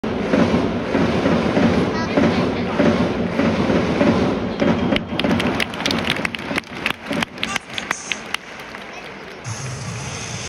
巨人、戸郷翔征のピッチング
2022/09/17
巨人対阪神
巨人、戸郷翔征、
原口から三振を奪う。